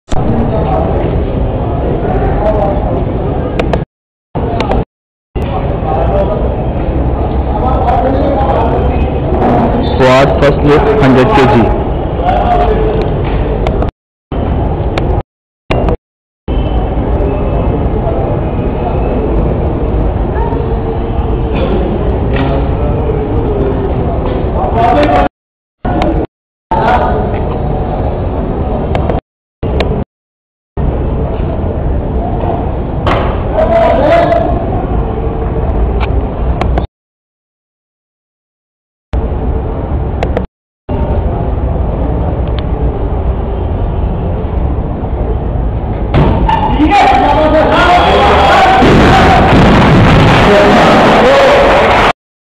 Pakistan Weightlifting
Fawad Mirza--Lifting 100kg Snatch at Inter-Colagiate Weightlifting Championship 2010